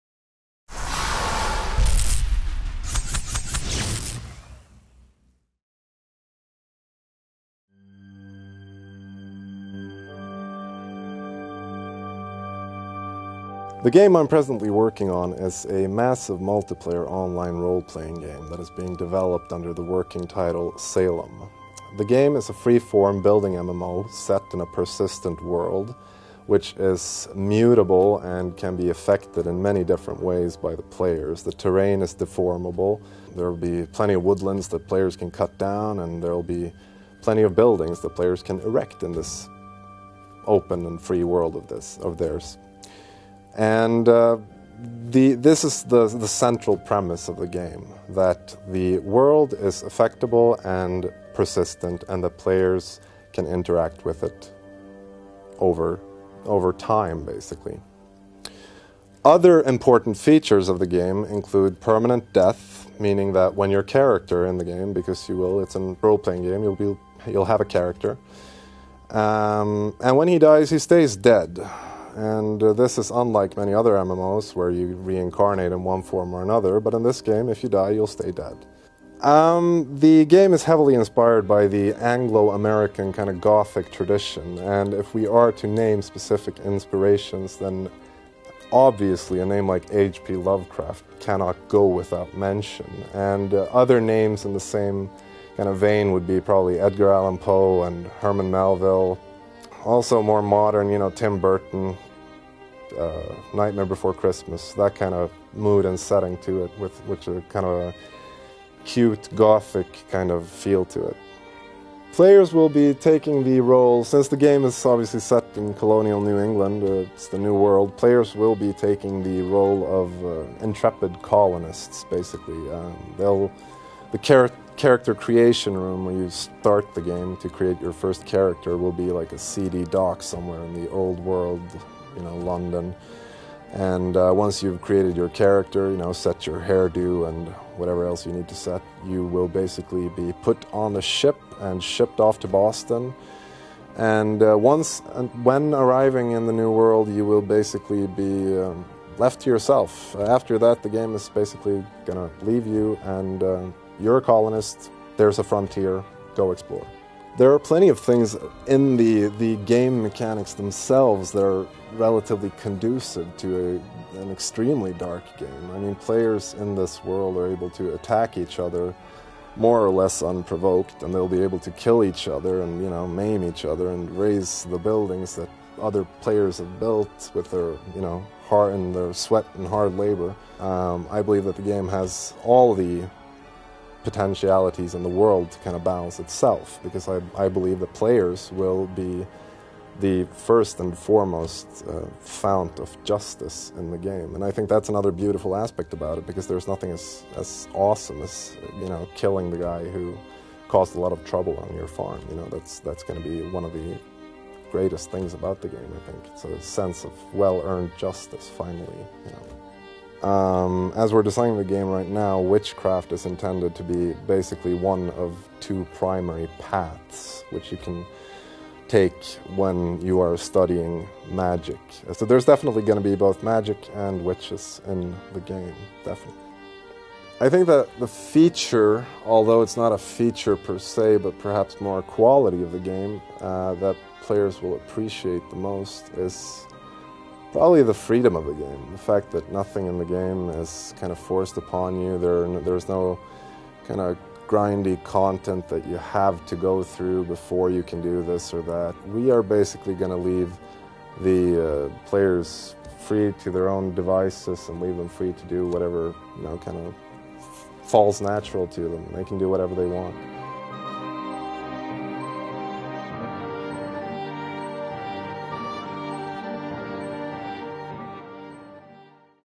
Salem Interview with Bjorn Johannessen
An interview with Salem's Game Designer Bjorn Johannessen, talking about permanent death in the forthcoming colonist MMORPG. Read more about Salem and other announced Paradox Games in our live-blog